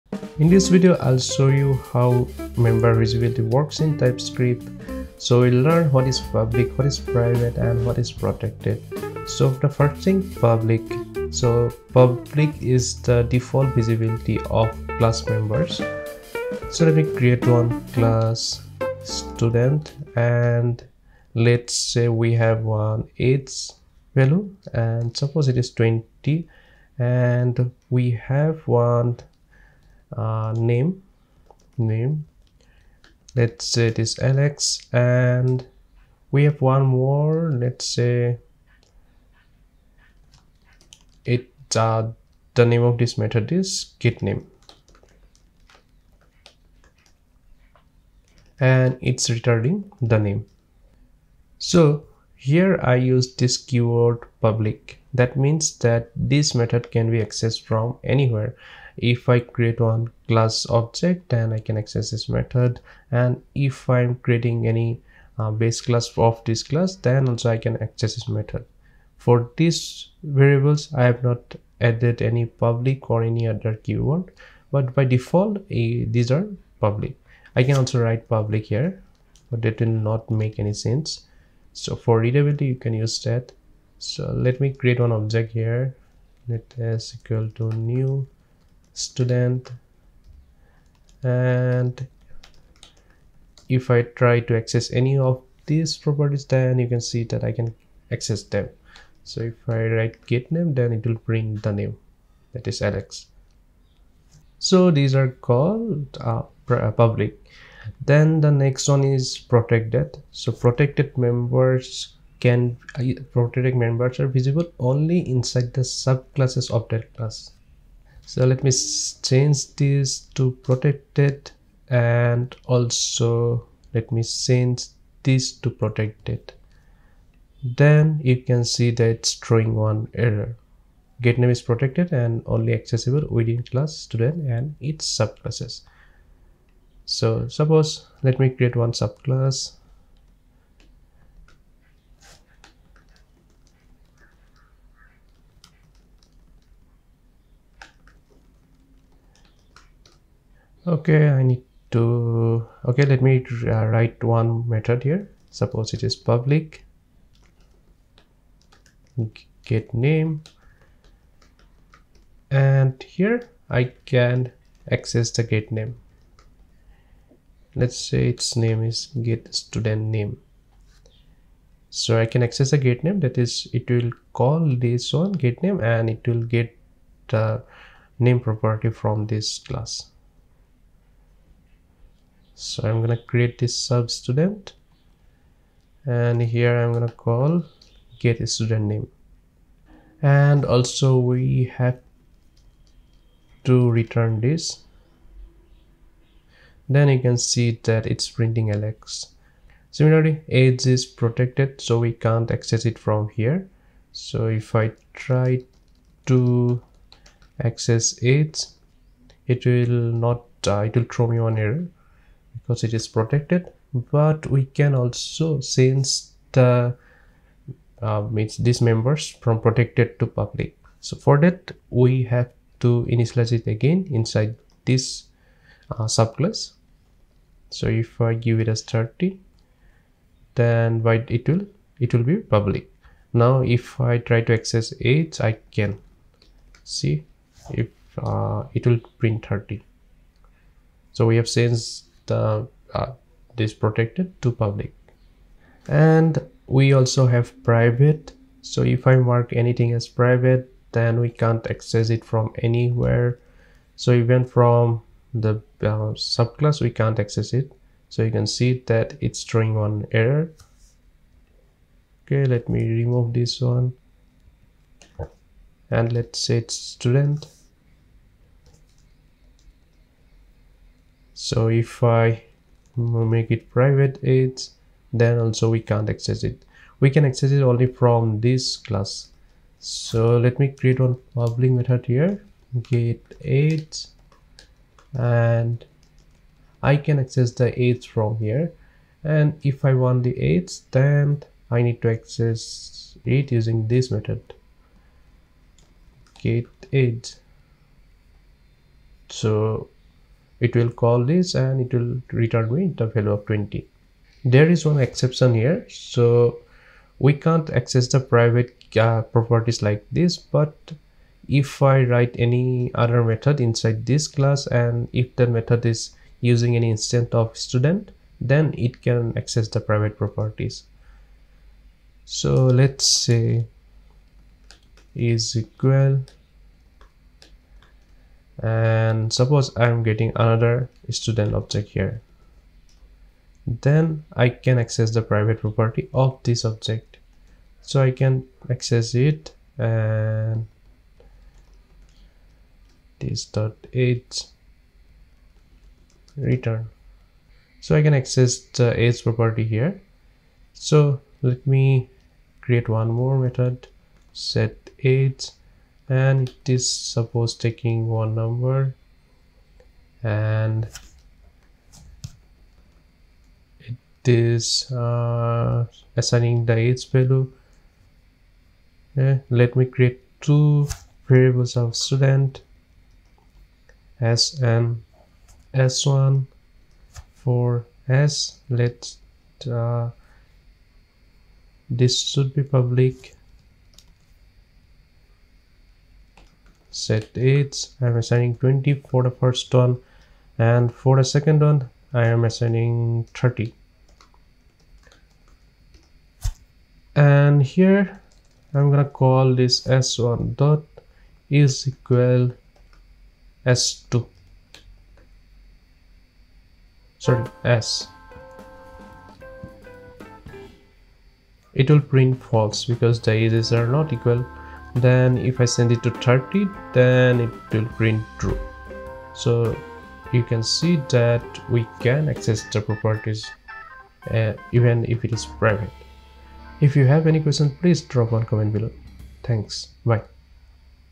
TypeScript tutorial 16: public, protected and private properties in TypeScript | Learn TypeScript
Learn how to use public, protected and private properties in TypeScript. This video will show you public, protected and private member variables and methods in TypeScript with examples.